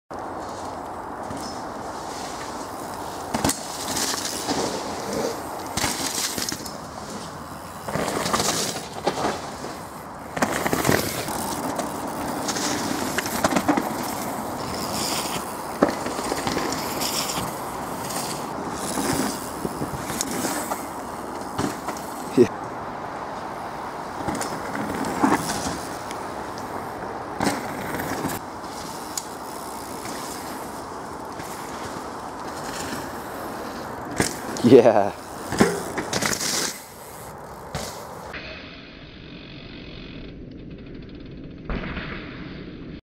Quick line from Edenfield
Just a short video from a nice easy line at Edenfield woods. We did plan to make a full length vlog but the lighting was terrible and this is the best we could do with the conditions. :)